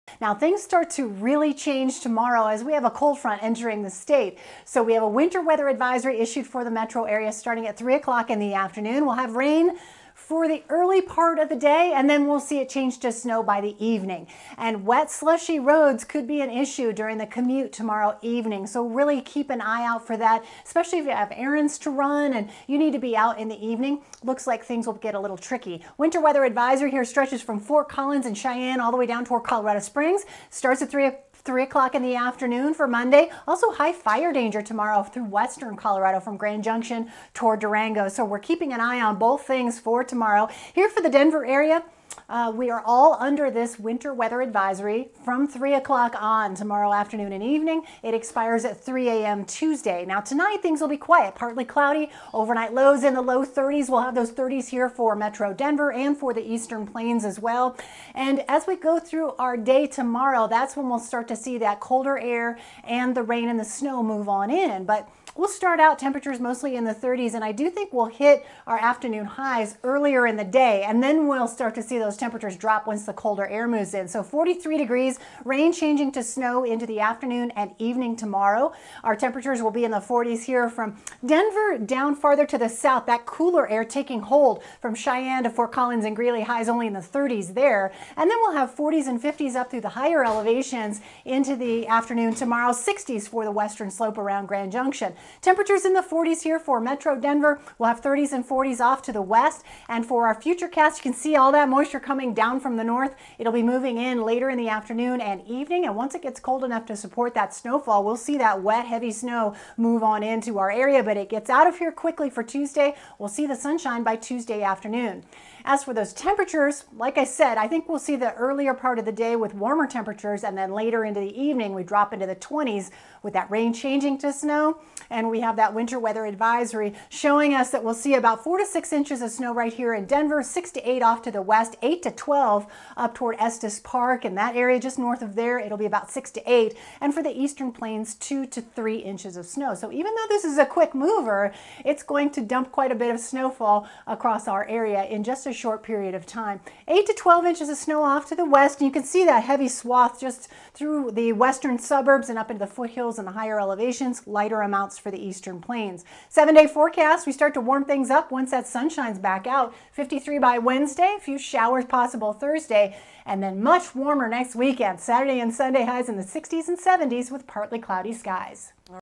Sunday night forecast: Snow for Monday
A Winter Weather Advisory on Monday for Denver- snow by the evening.